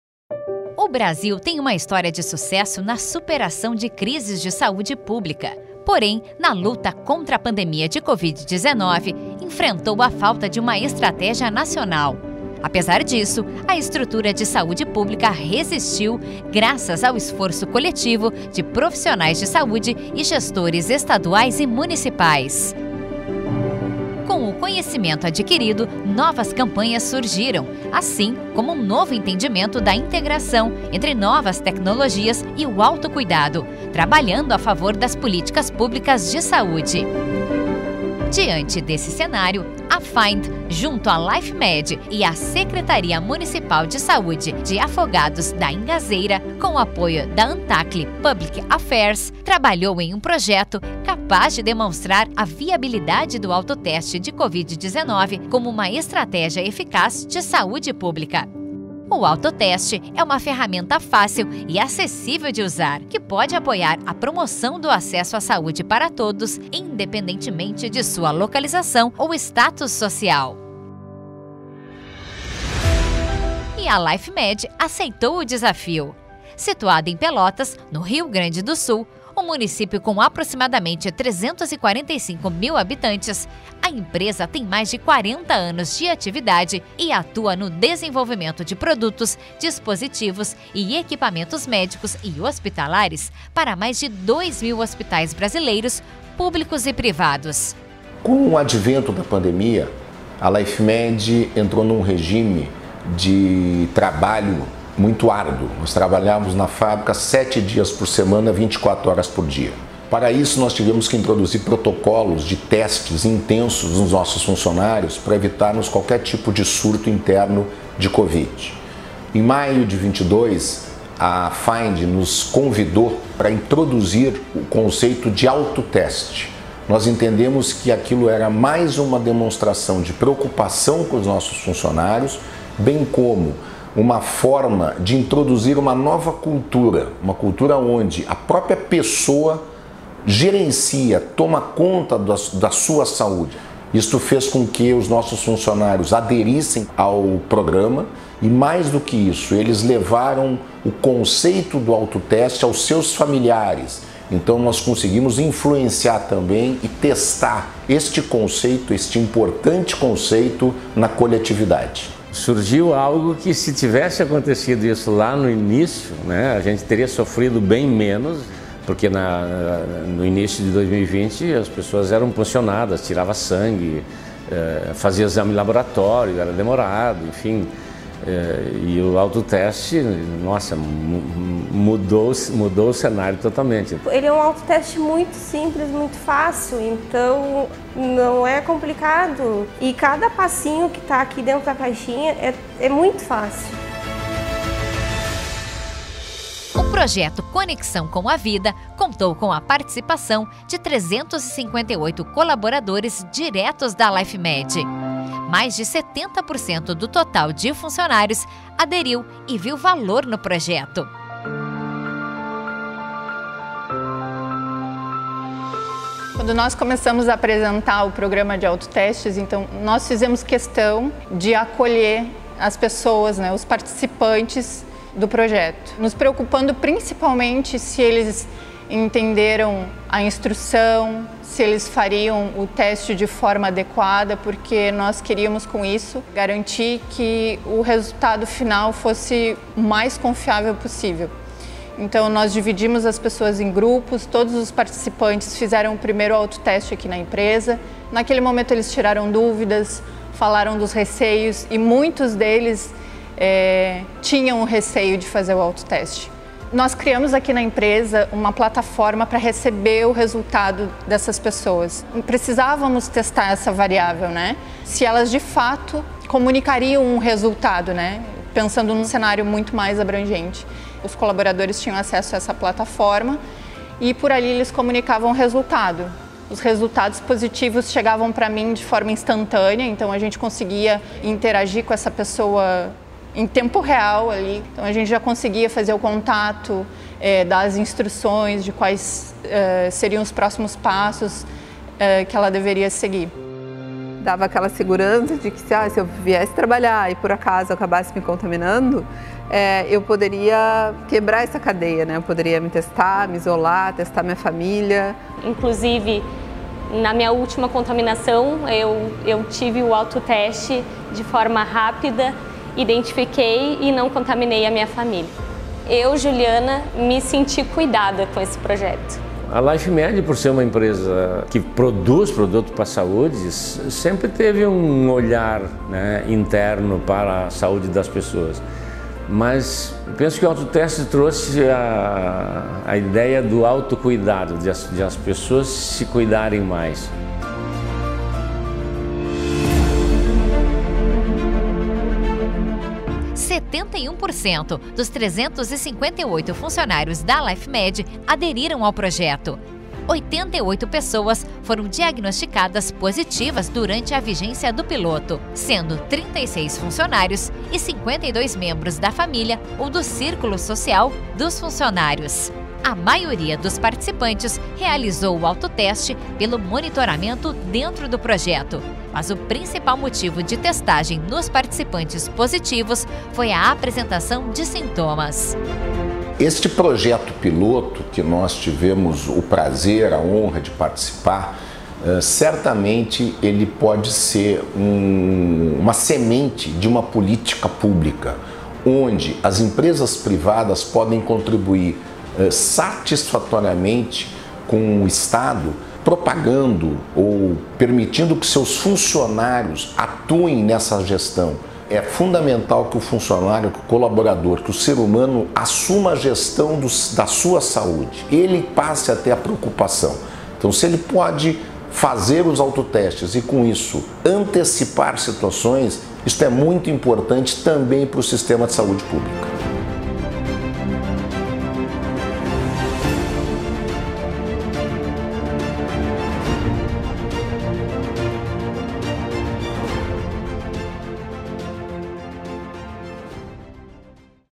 Self-testing & self-care: promoting a holistic approach to employee health in Brazil (long version)
Self-testing & self-care. Optimization of COVID-19 self-testing service delivery strategies to different contexts and populations is key to reduce transmission and improve access to care. FIND and Lifemed in Pelotas, Brazil, with the support from Antakly, have implemented an innovative self-testing model to increase the access to diagnostics for employees and their household members, and reduce COVID-19 transmission in the workplace. More than 250 employees have participated in this pilot, which is being expanded to a more holistic employee health program that builds on self-testing and self-care.